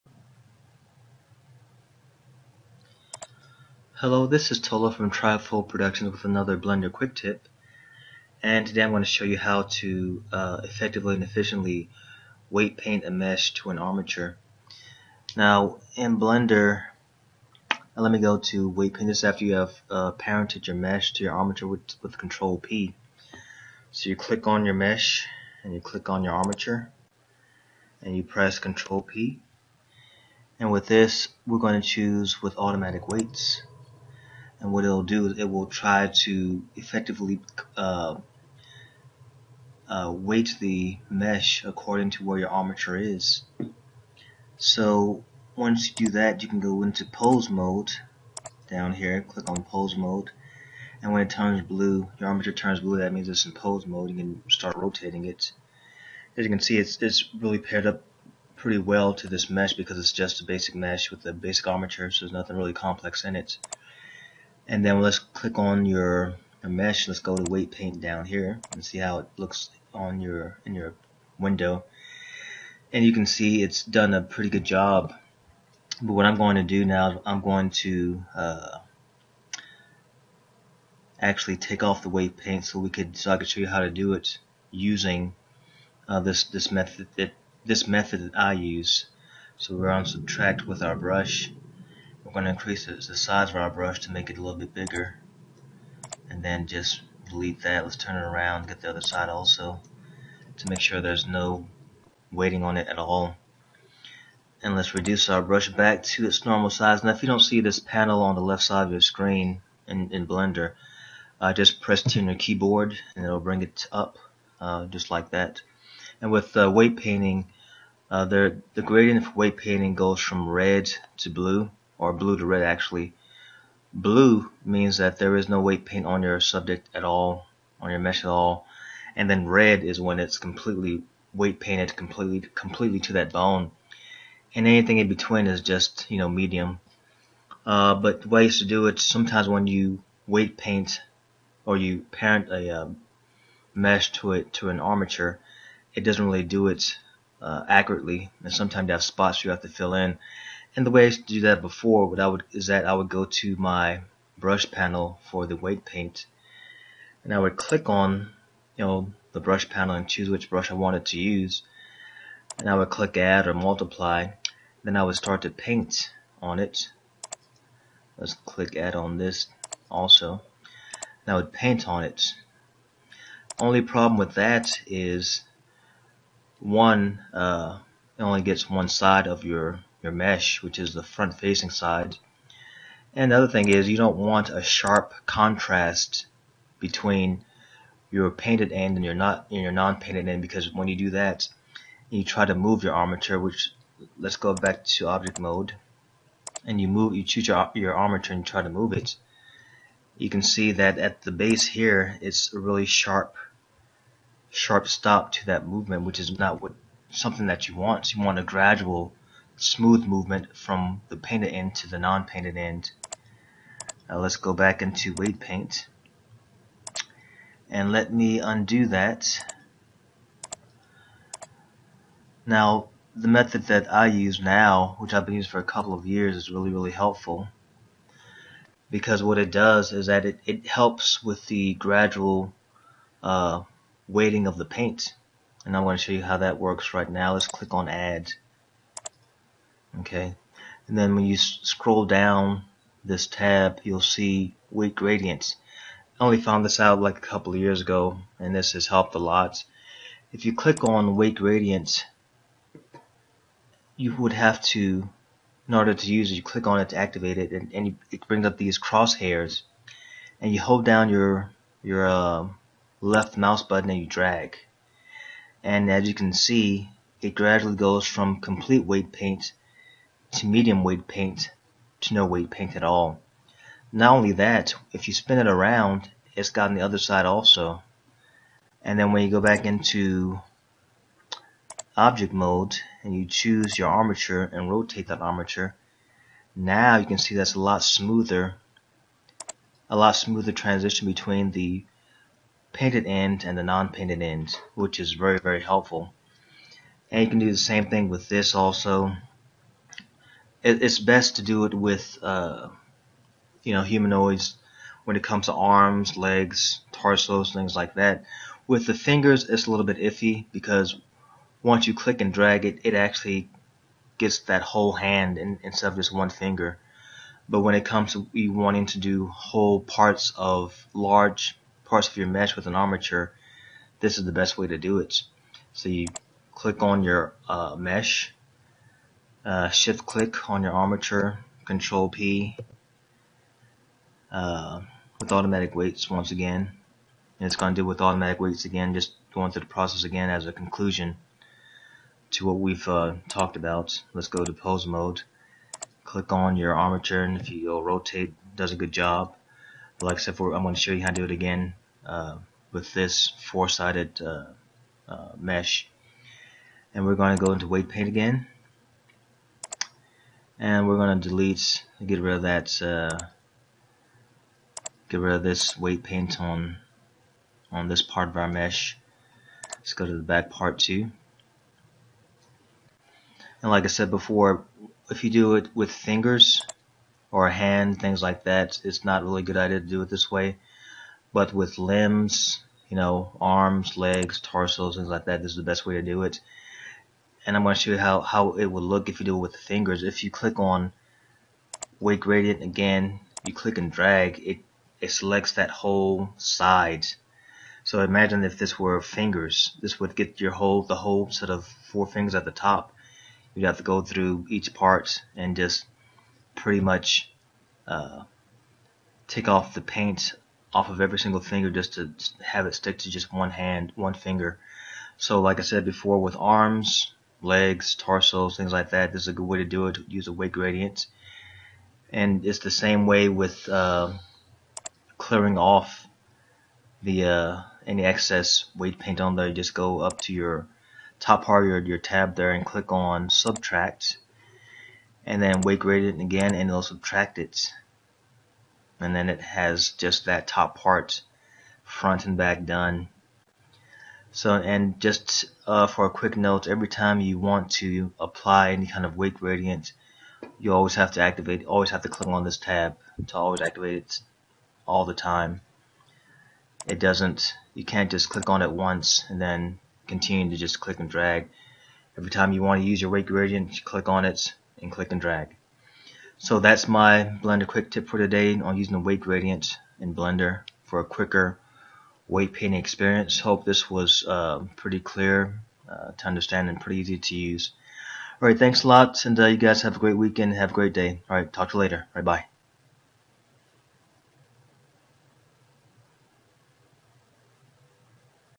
Blender Quick tip: Using the weight paint gradient
In this tutorial, you'll learn how to use the weight paint gradient to effectively weight paint a mesh to an armature.